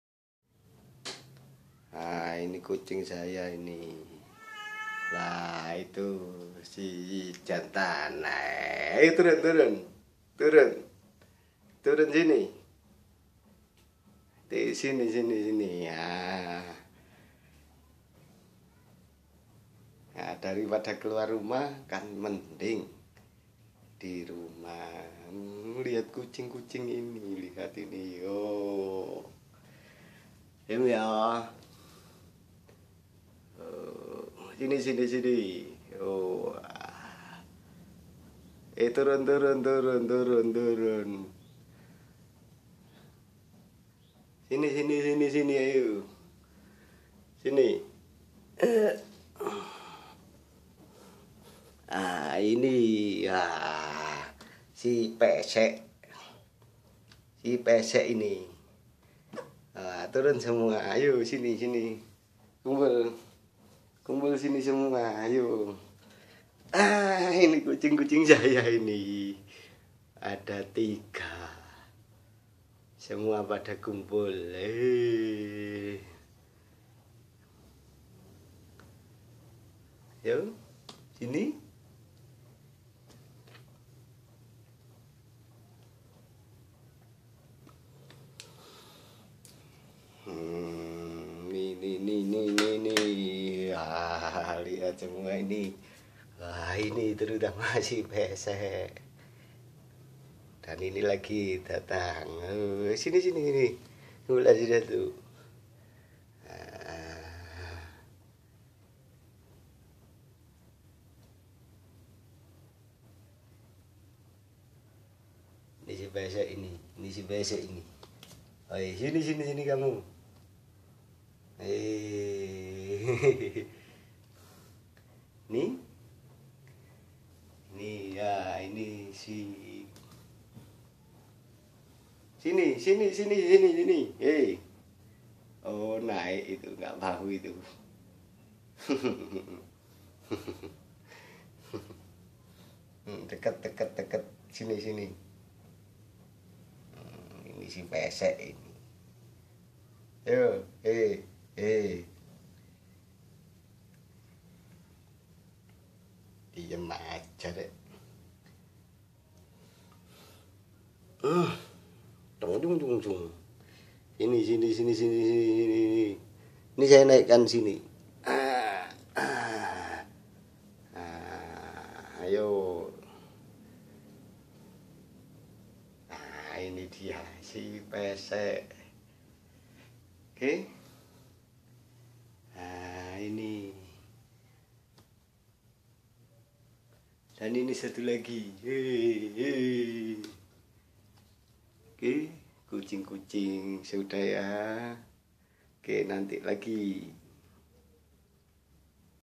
Liburan dirumah sama kucing-kucing saya
Tidak keluar rumah untuk mencegah agar terjauh dari wabah Corona.
Semoga kita semua diberikan ketabahan menghadapi semua ini dan segera dibebaskan dari wabah ini.